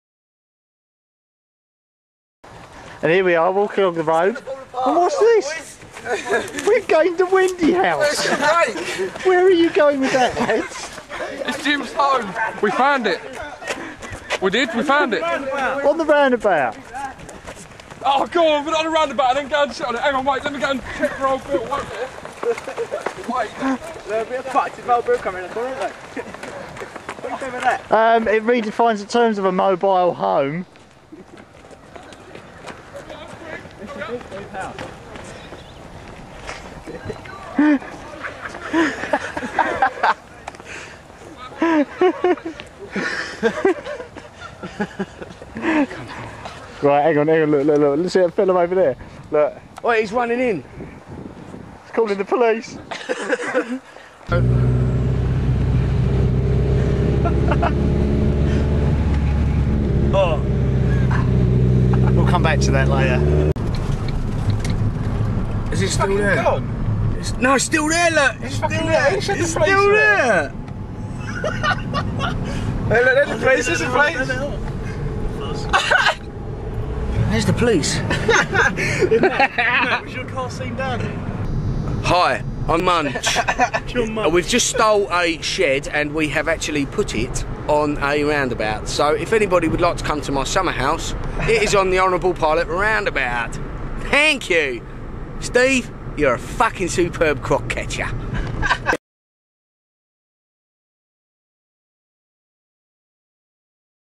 Munch's summer house or doing a tramp a favour? shed!!!
Yes it seems I was walking along the street when all of a sudden this shed just wanders past. seemingly as a donation from a local retailer...